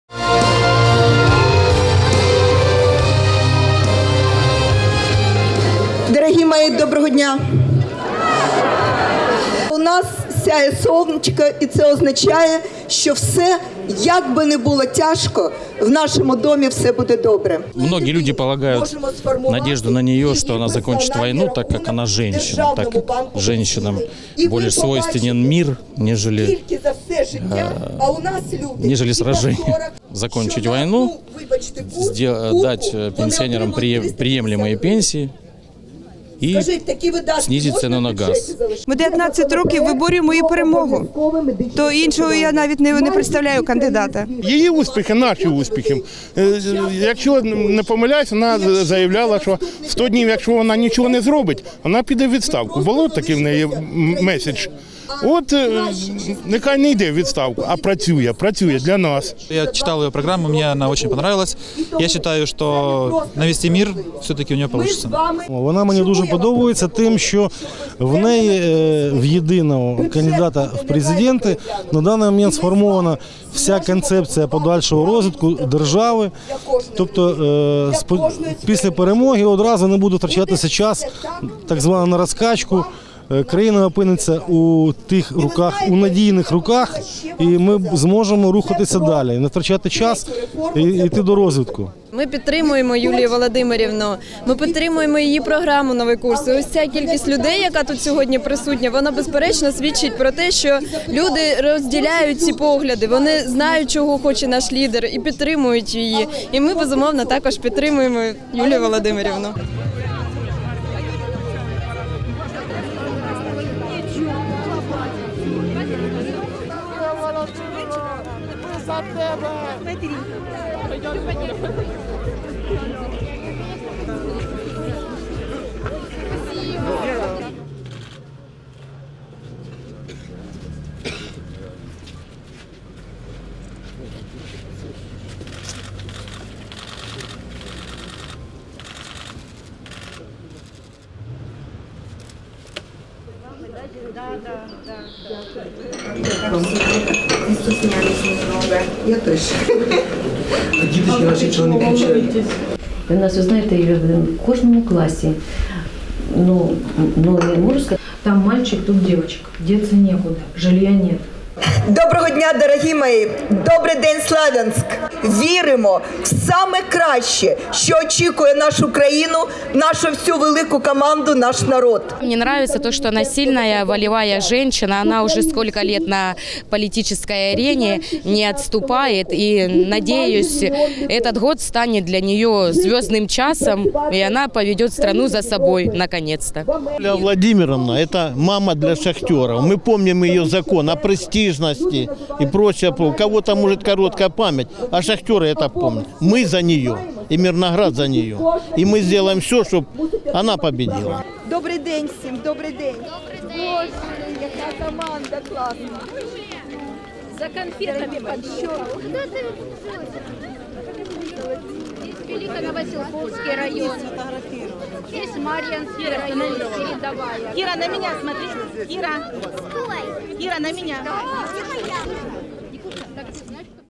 Юлія Тимошенко у Донецькій та Луганській обл. 17-18 березня 2019 р.
Юлія Тимошенко 17 і 18 березня 2019 року відвідала Донецьку та Луганську області в межах всеукраїнського туру. Вона провела 10-тисячну зустріч із людьми у Сєвєродонецьку, зустріч із переселенцями у Святогірську, мітинг у Слов’янську. Повернути мир, відродити Донбас, захистити переселенців - такі завдання кандидат на пост Президента України Юлія Тимошенко ставить собі як пріоритетні.